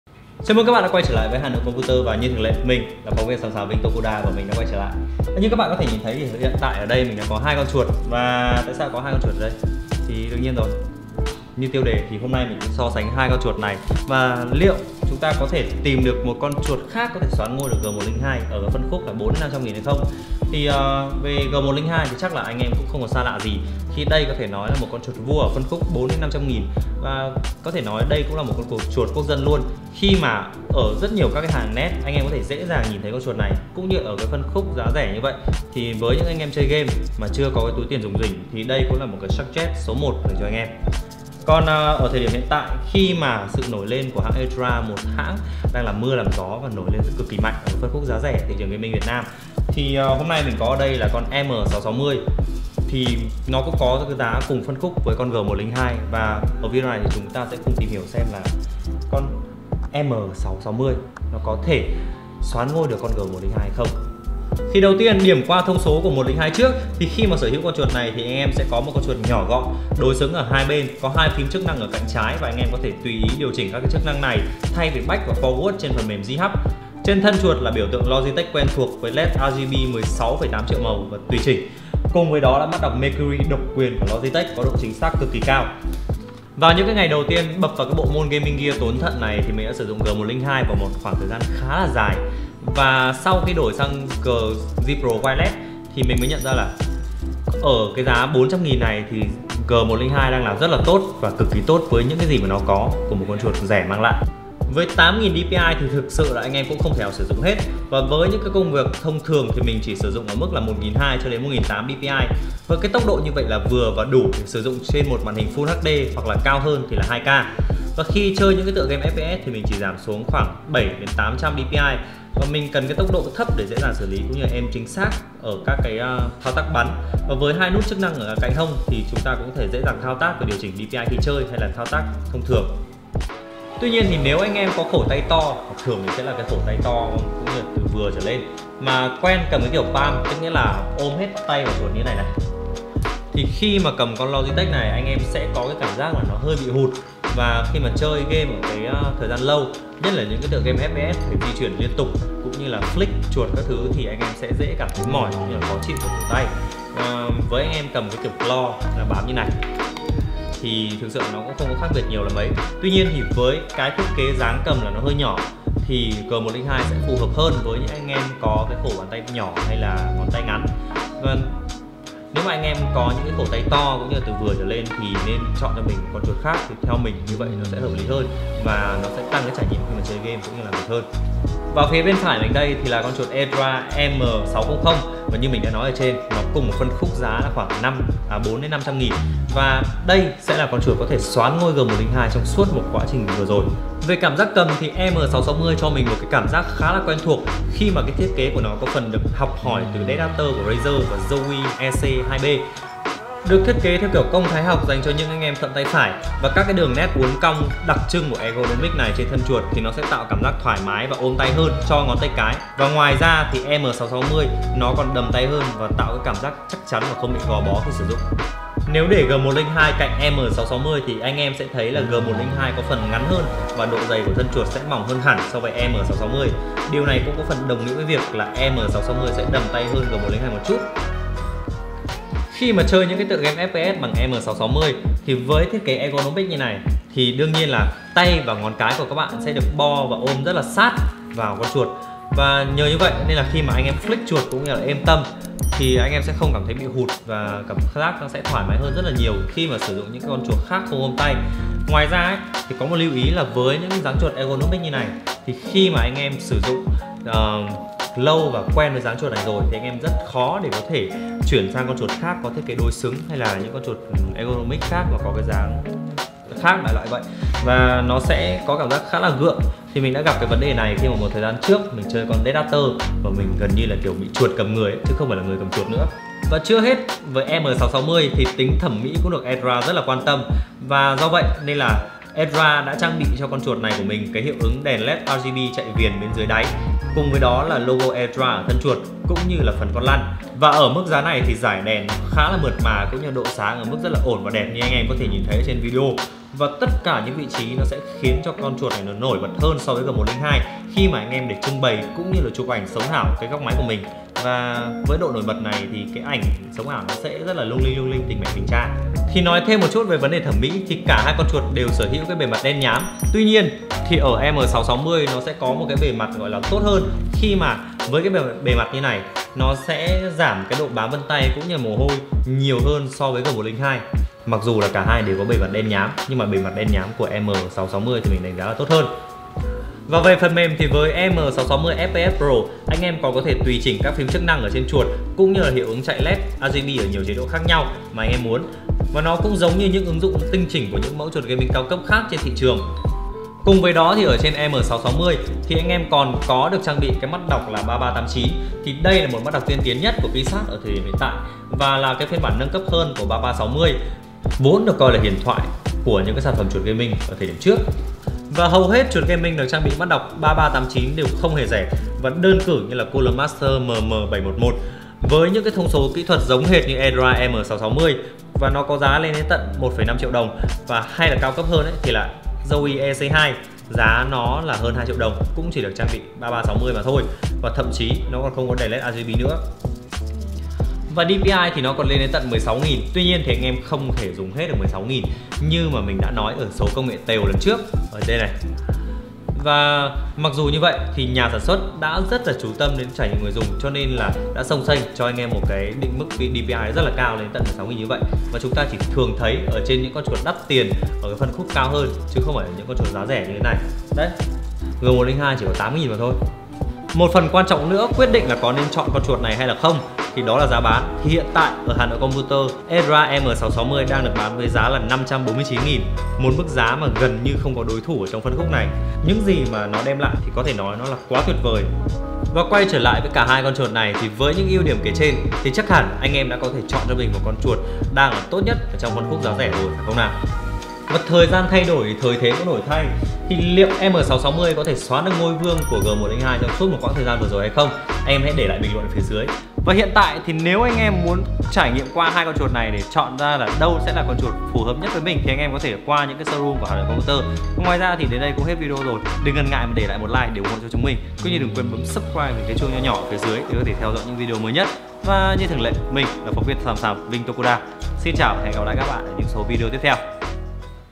Đi Tìm Ngôi vương GAMING MOUSE giá rẻ!!! - G102 vs EM660!
Trong phân khúc chuột gaming giá rẻ chắc hẳn các bạn đã không còn lạ lẫm gì với em chuột G102, nhưng hôm nay Hanoicomputer mang đến cho các bạn một ngôi vương mới trong chuột gaming giá rẻ, đó là E-dra EM660,  hãy cùng khám phá và so sánh 2 em chuột này ngay thôi nào!!!